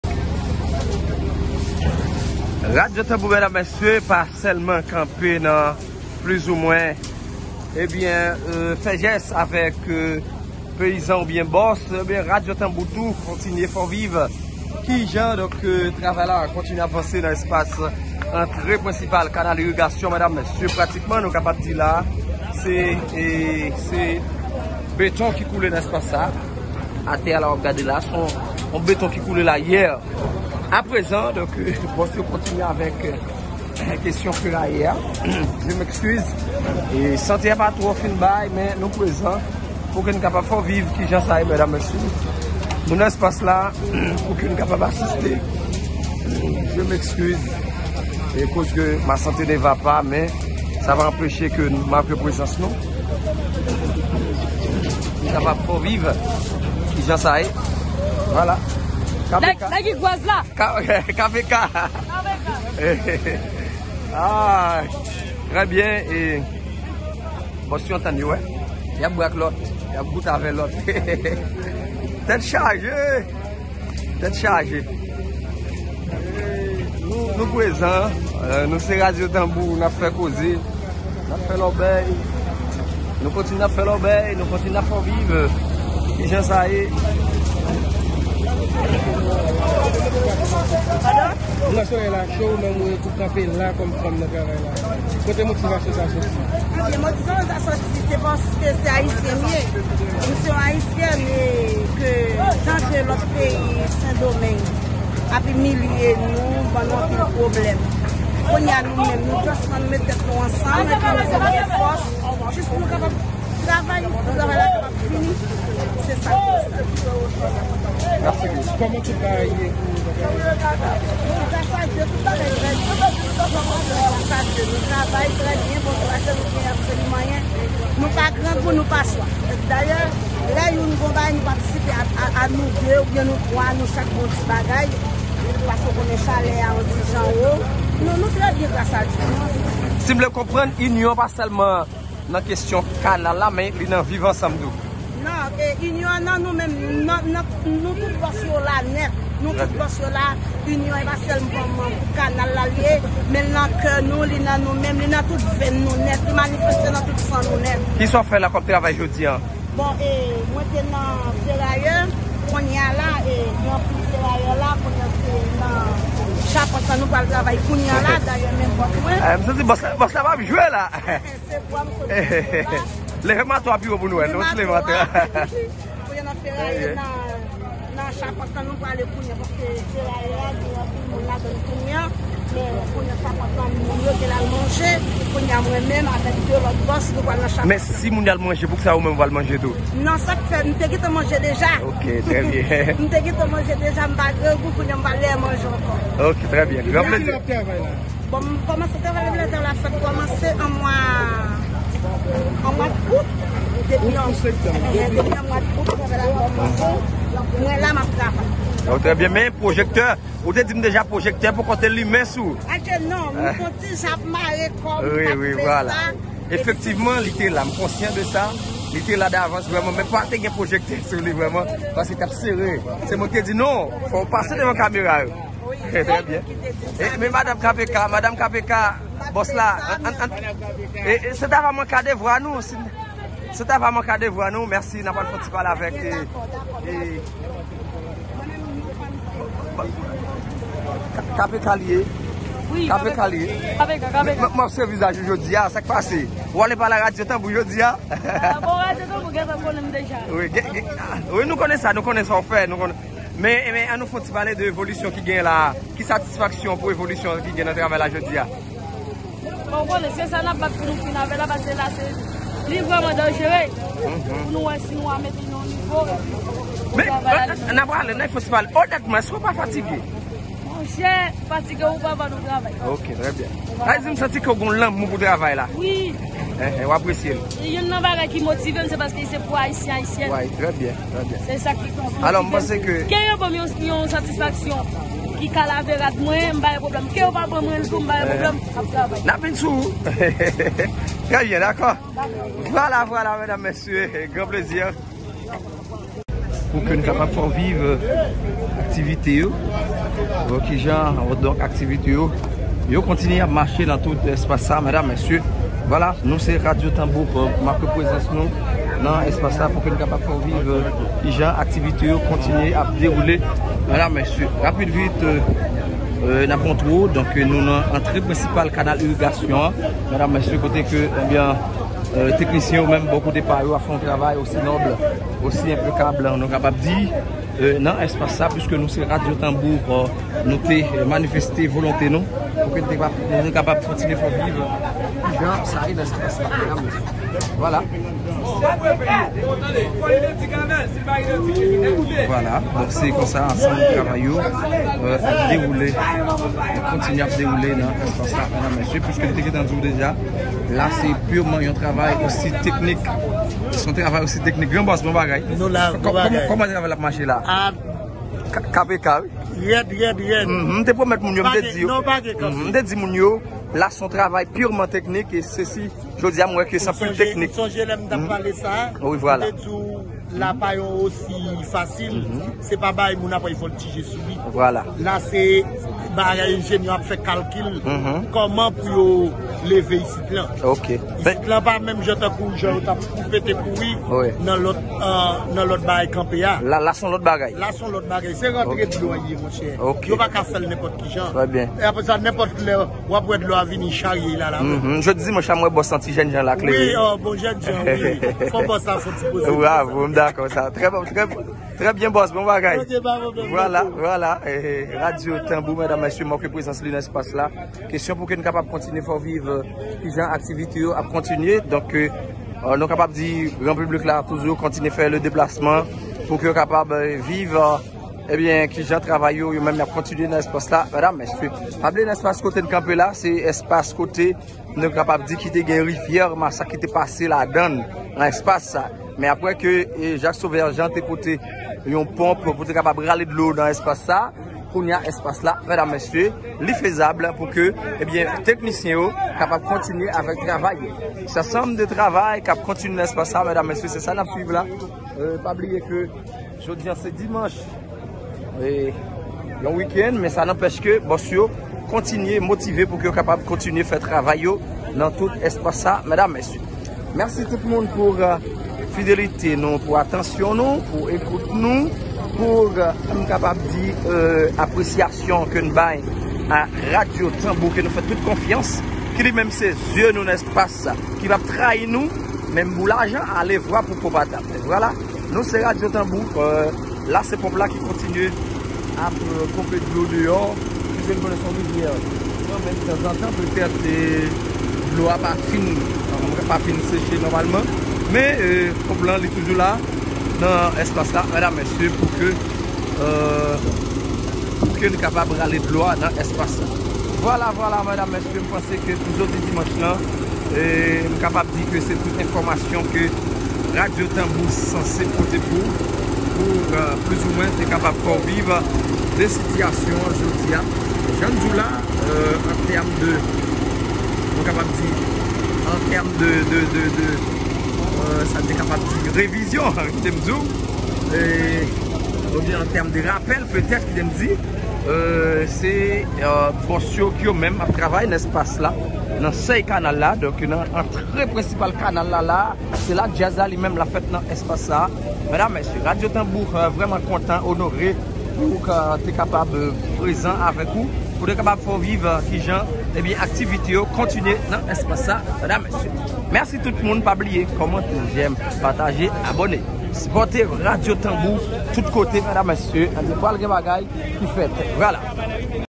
MEN KÒMAN TRAVAL KANAL LA TAP MACHE JODI LUNDI 30 OKTÒB LA. kpk kanal.
Abòne ak chanèl @RadioTambouATL la pou tout ti detay de sa kap pase sou kanal la